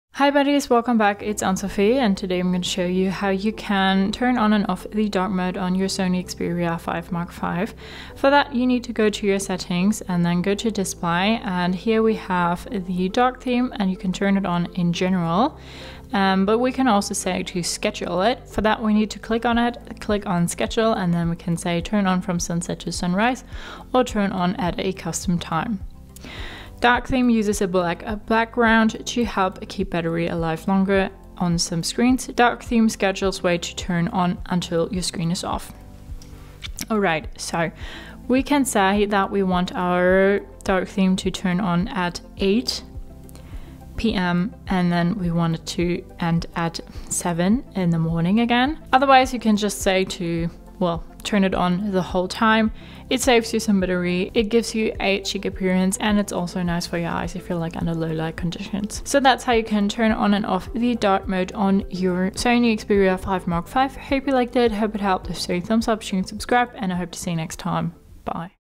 Sony Xperia 5 V - How to enable Dark Mode • 📱 • ⚫️ • 😎 • Tutorial
Do you want to know how and where you can turn on the dark mode on the Sony Xperia 5 V? In most cases it will save you some battery, is good for your eyes when you are under low-light conditions and honestly... it gives your phone a pretty chic appearance!
In this video we will show you where you find your dark mode setting, how you can enable and disable the dark mode and what else you can do with the dark mode.
Side Note, some apps sadly DO NOT support the dark mode.
Have fun with your new Xperia 5V and the video! 🙌🏼